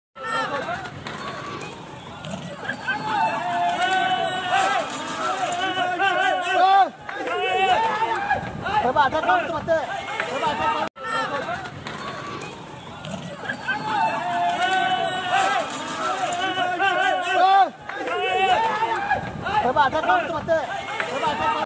Leopard runs in panic as people chase it in Mysuru. Rescued by forest officials
A video of a leopard trying to escape humans chasing it surfaced on social media and it is going viral. The video is reportedly from Mysuru in Karnataka. The leopard was safely tranquilised by forest department officials later.